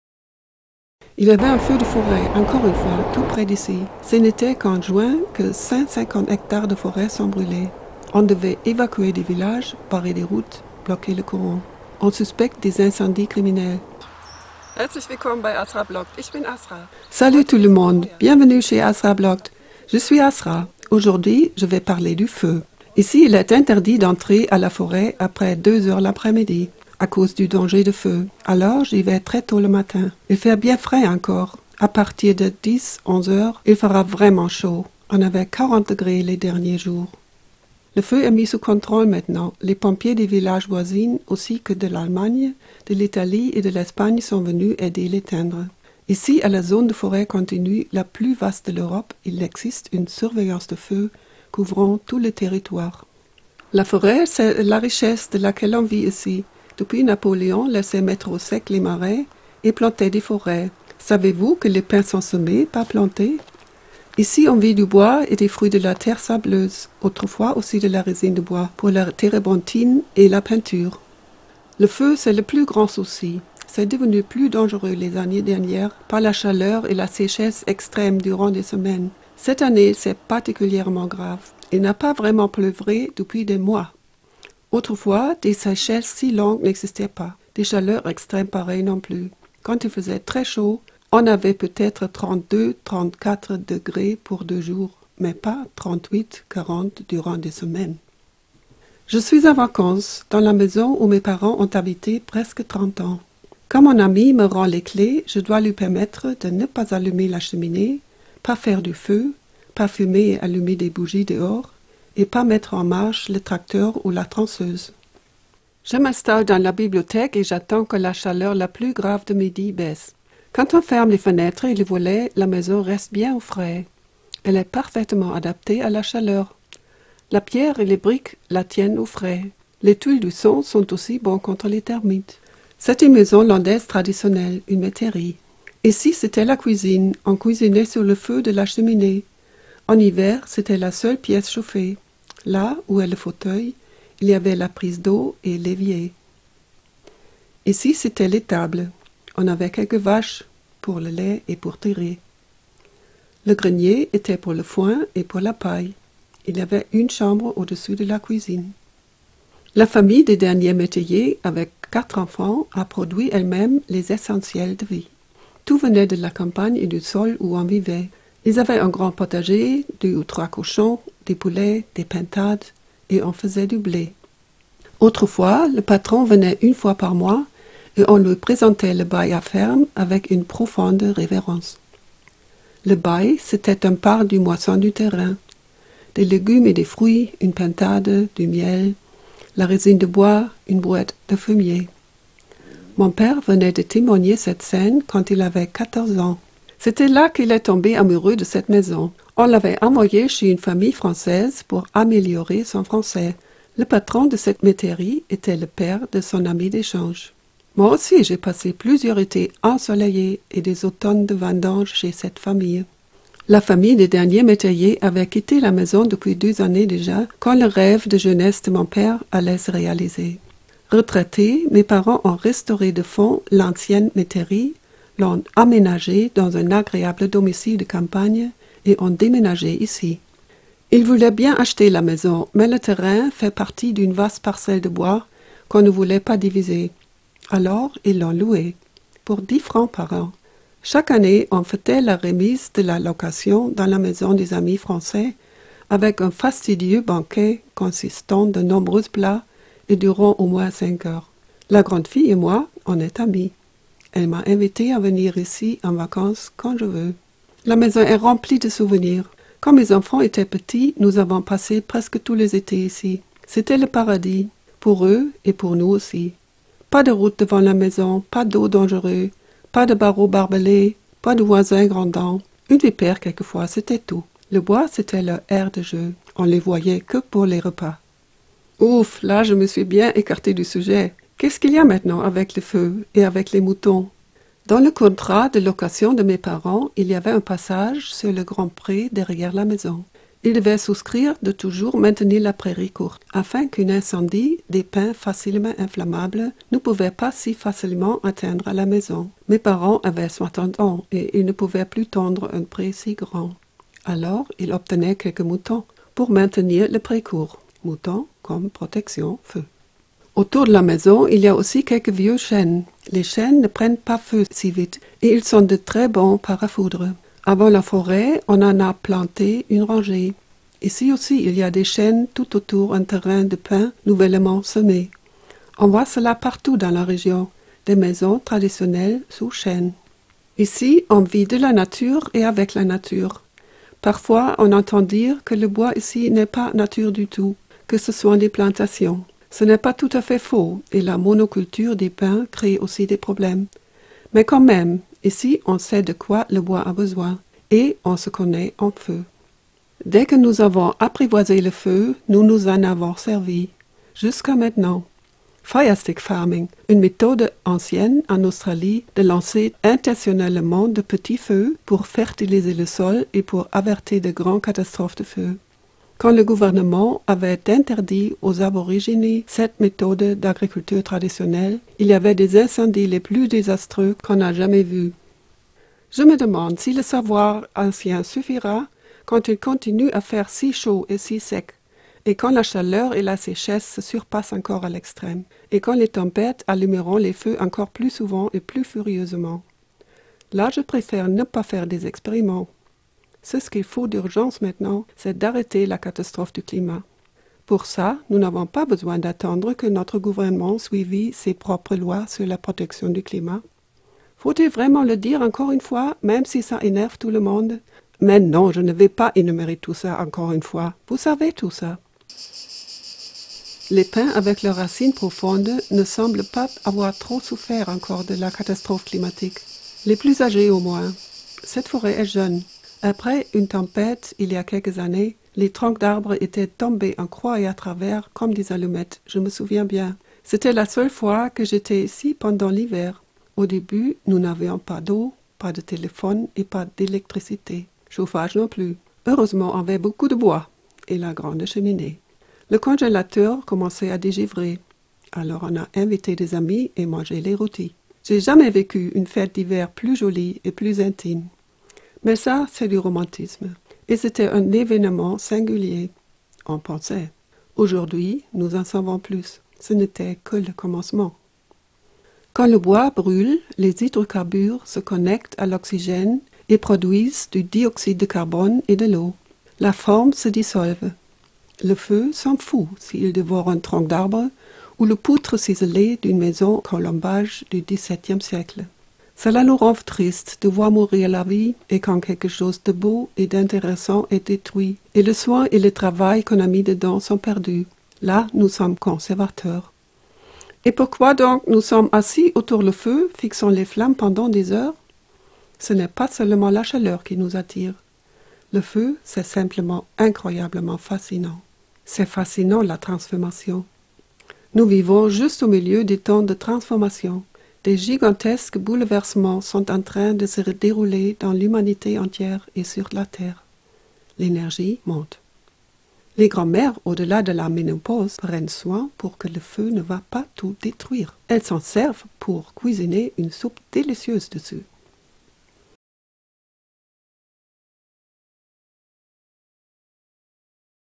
Feu et Moutons
Si on habite dans la forêt, le feu est le plus grand danger. Autrefois, on savait comment protéger sa maison contre le feu. Avec la chaleur, la séchesse et les tempêtes extrèmes des dernières années, tout est devenu beaucoup plus dangereux. Comment des moutons peuvent aider à se proteczter contre le feu.